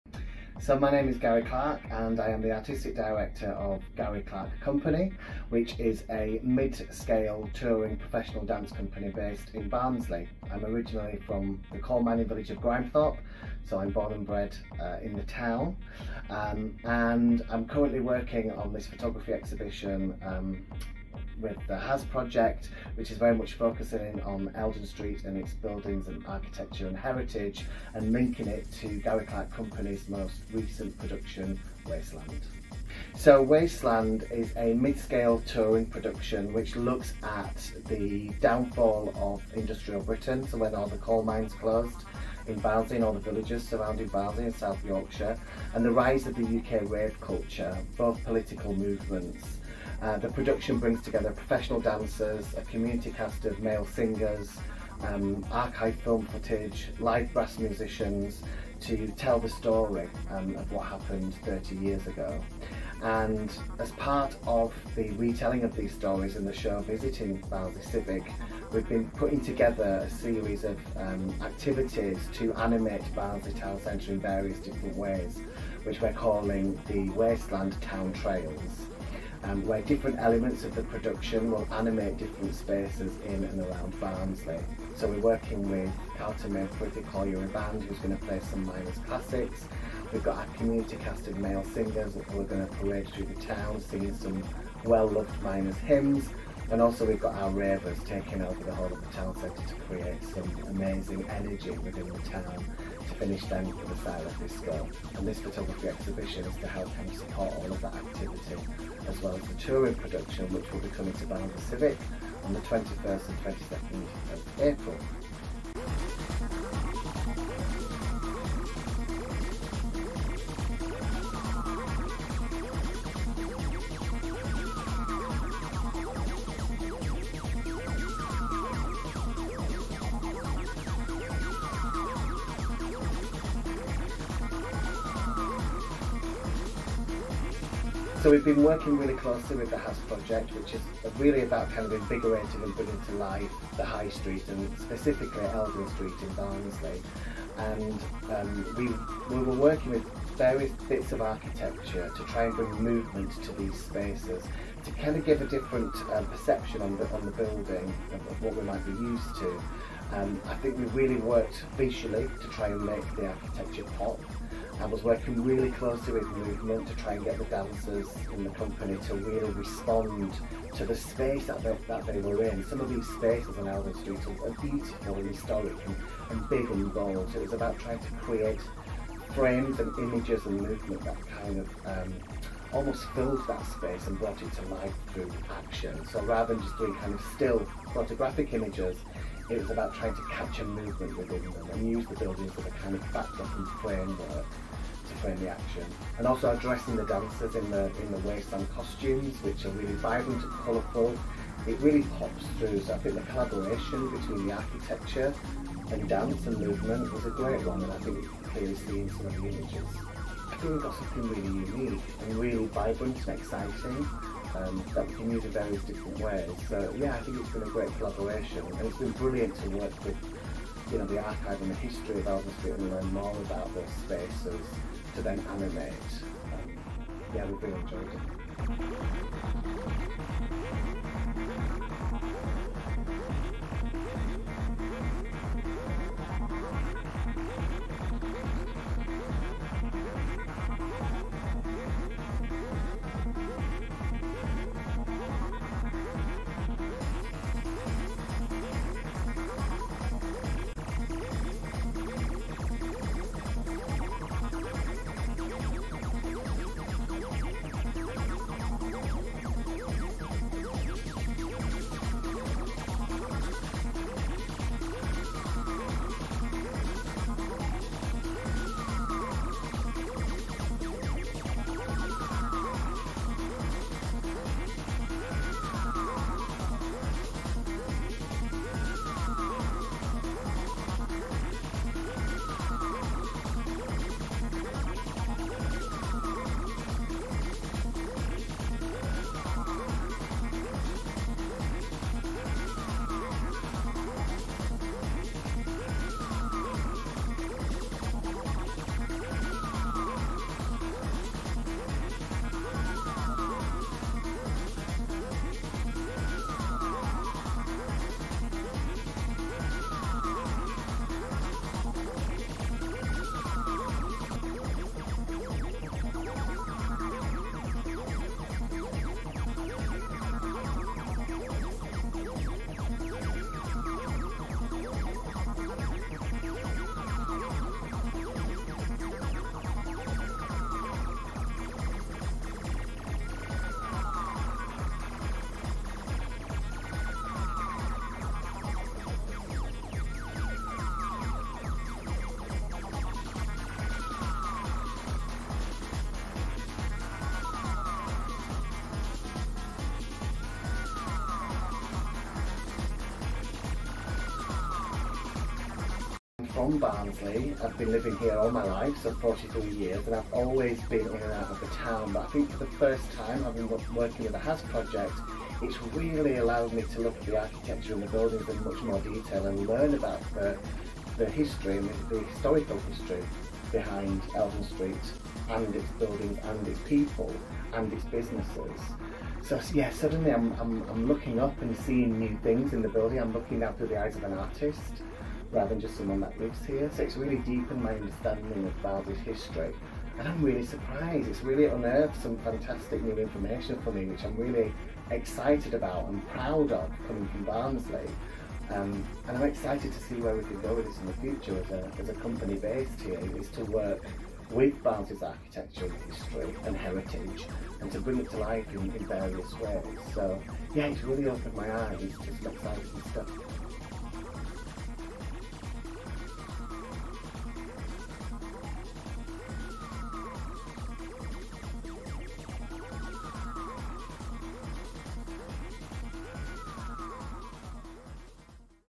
Gary Clarke Company and James Mulkeen Eldon Street photographic exhibition
Gary Clarke talks about working with photographer James Mulkeen to reframe and reimagine the historic spaces of Eldon Street in Barnsley through movement and modern dance as part of the Gary Clarke Company WASTELANDS town trails. Part of the Eldon Street High Street Heritage Action Zone. Created for a photographic exhibition at Experience Barnsley in April 2023 (running until the end of June 2023).